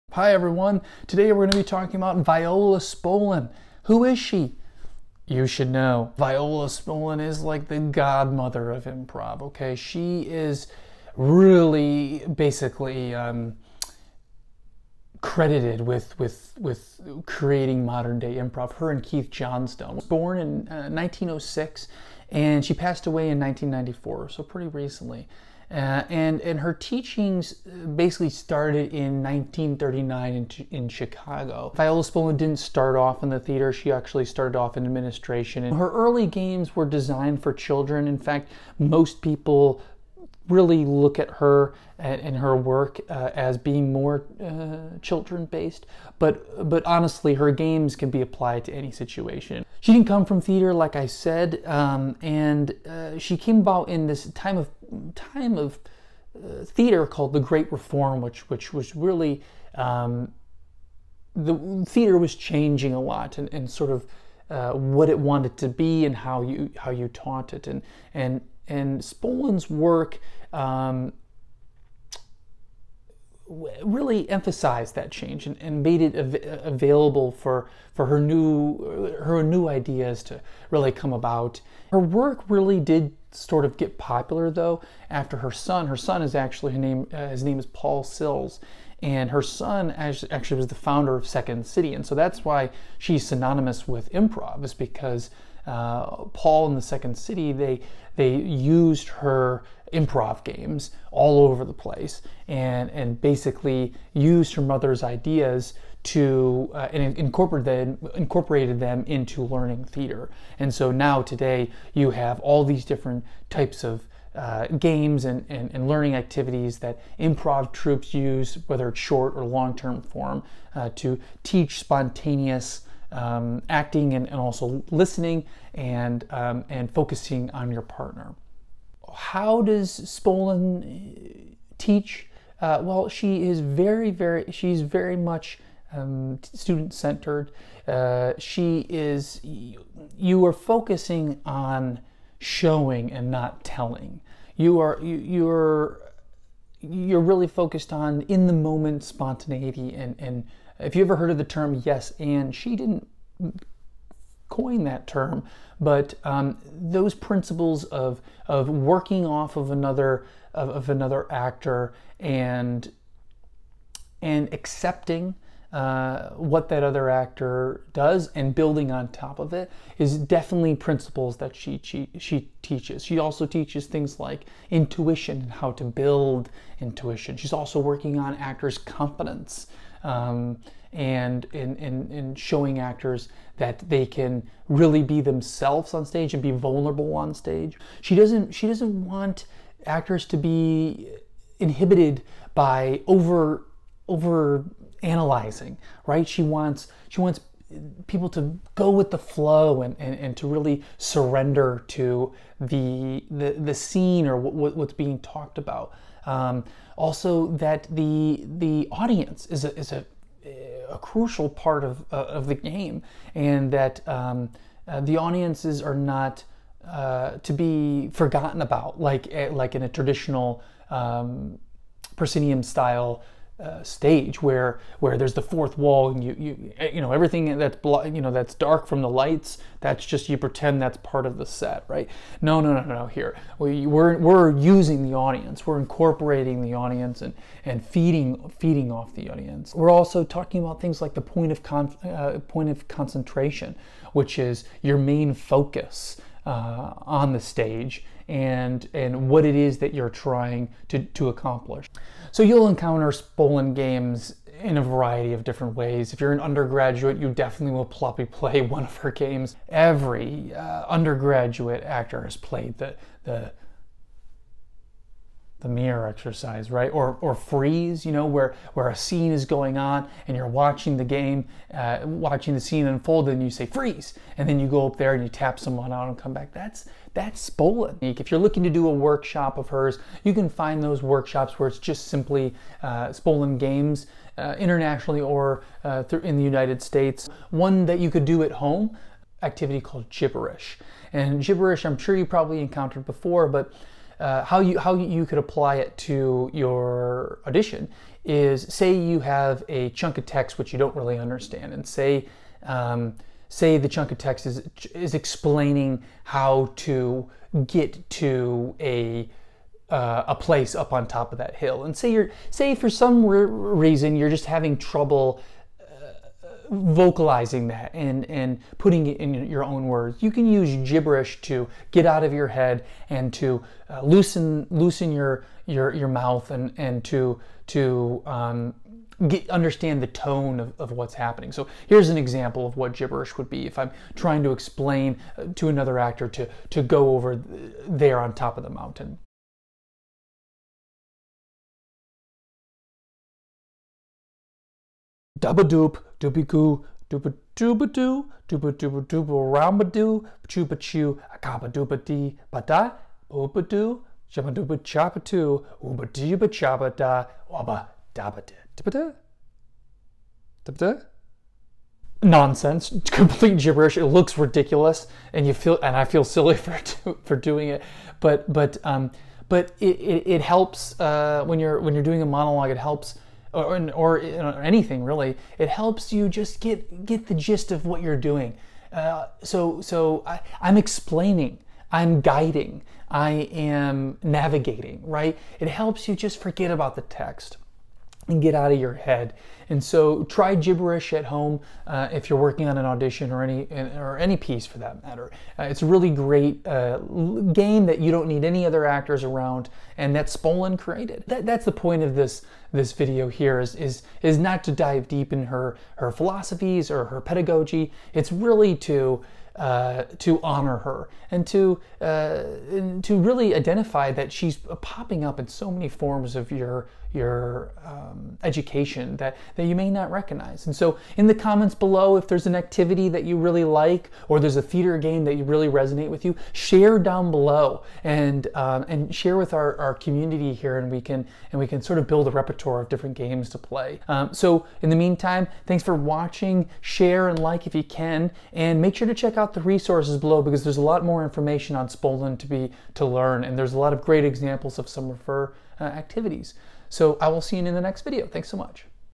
Who Is Viola Spolin?
Viola Spolin is the mother of all improv! If you don't know her name, watch this video to learn more about how she influenced improvisation as we know it today.

Works Cited/ Online Resources